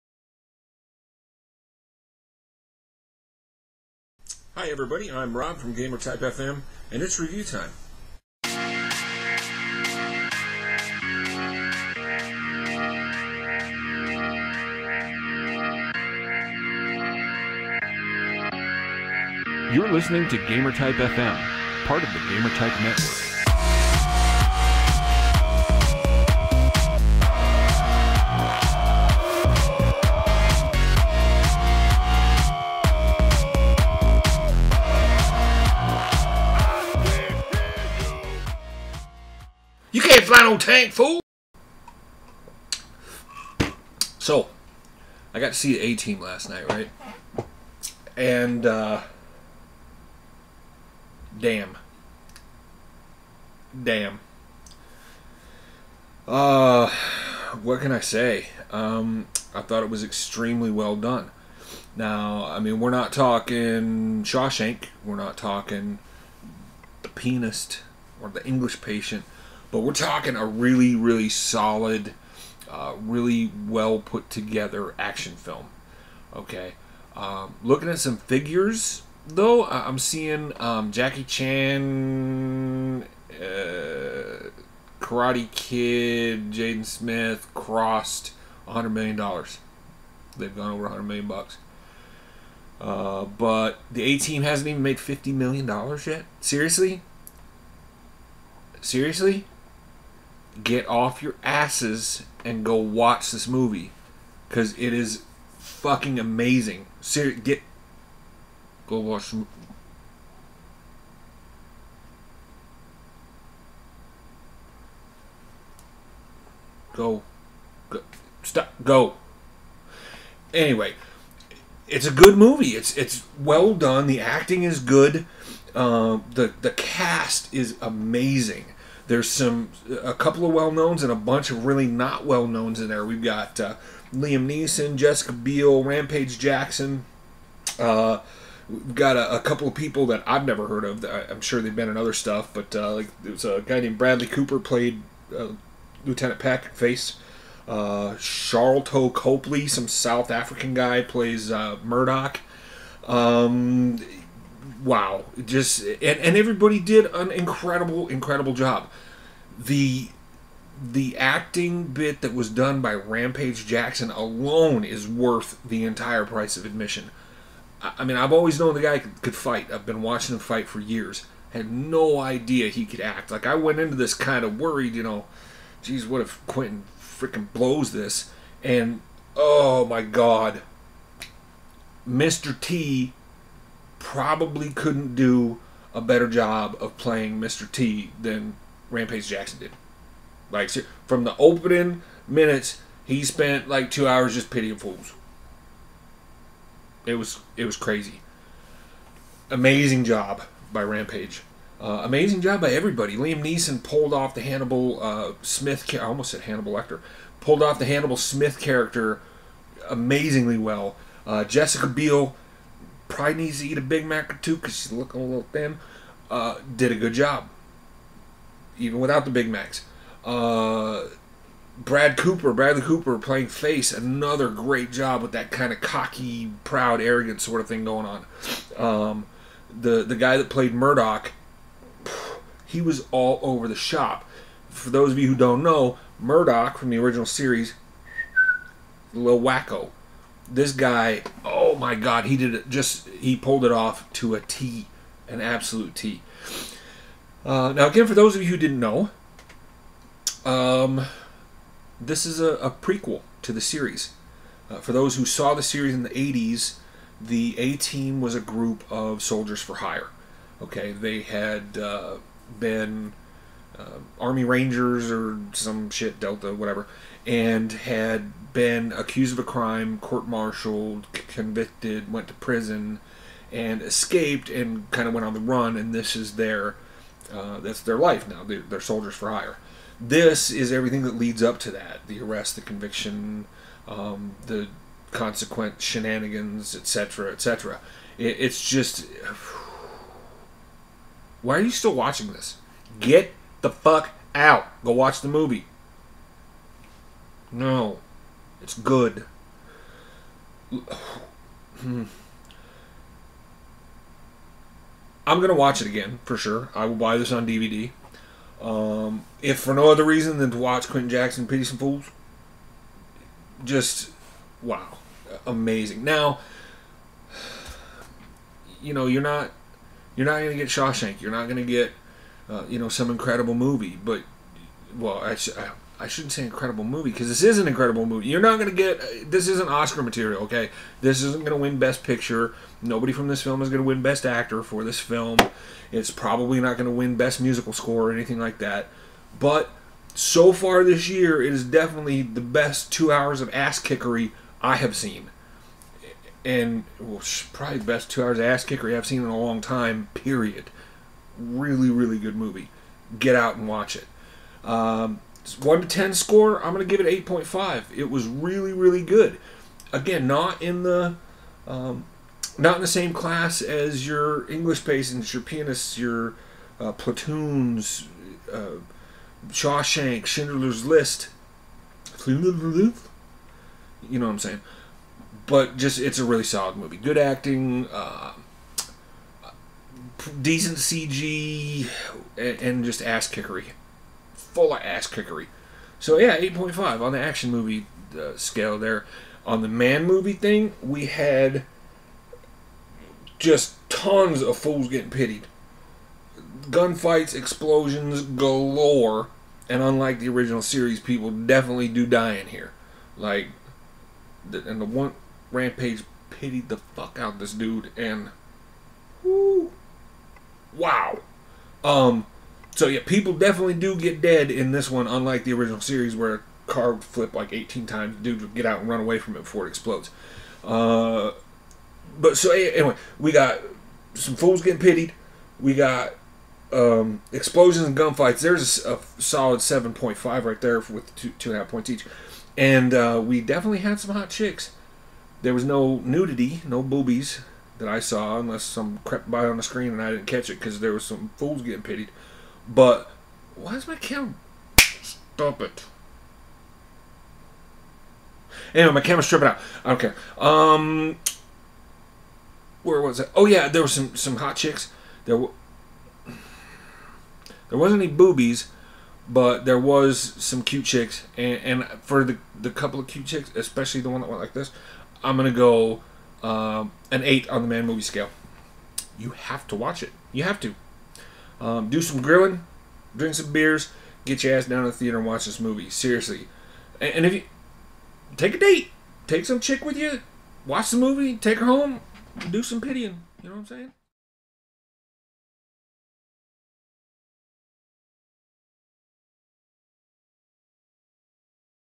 Rob reviews the new A-Team movie
This is an OFFICIAL GamerType FM movie review of the new A-Team movie.